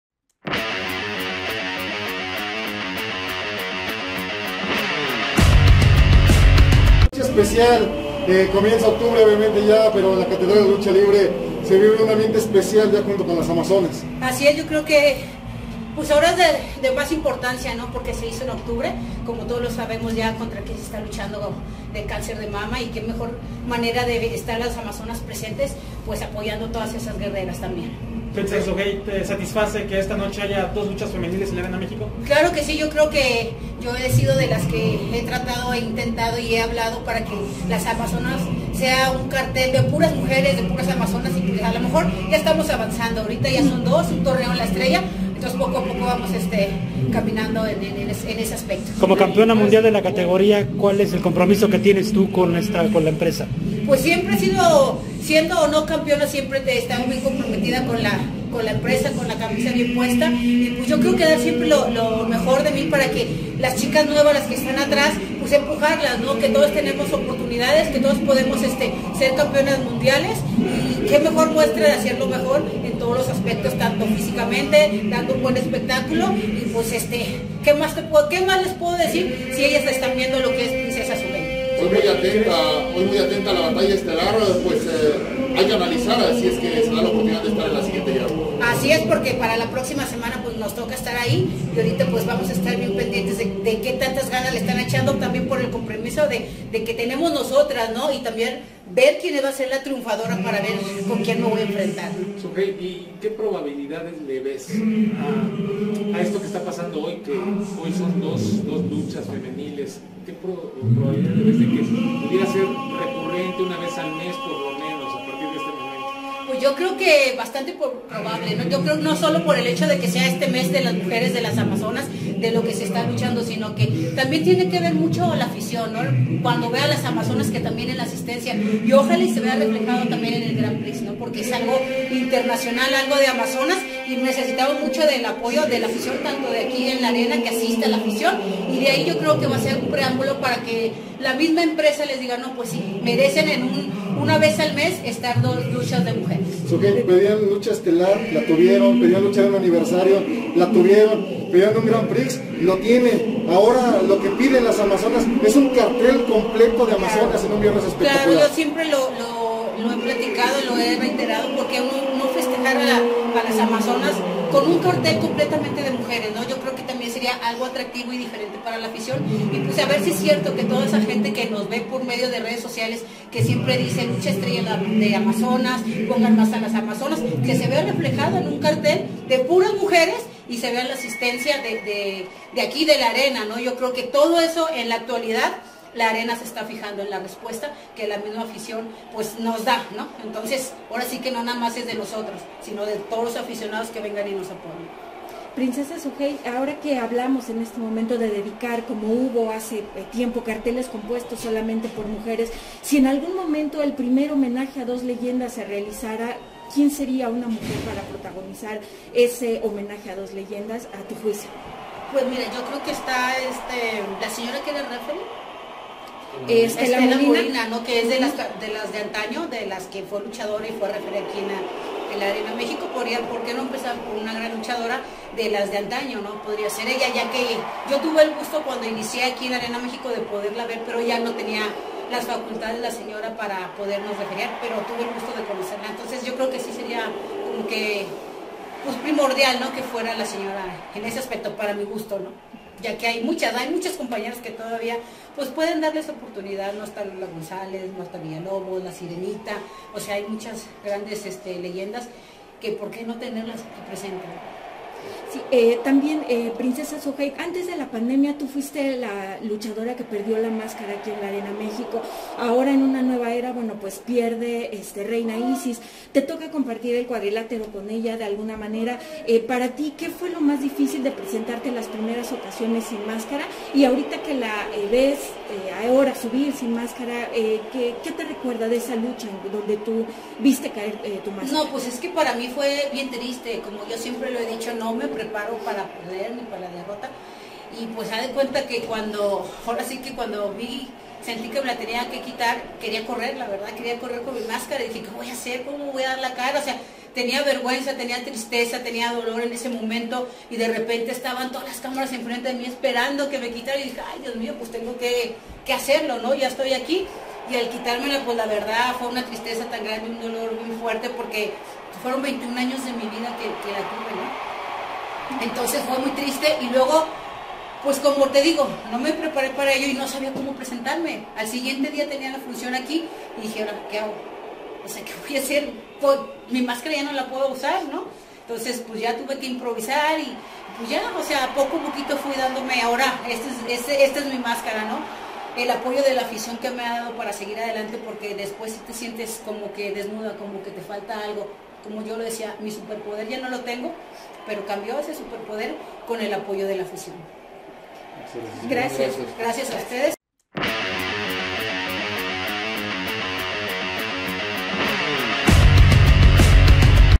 PRINCESA SUGEHIT "Espero un día haya un cartel en la Arena México de solo mujeres"
-CONTACTO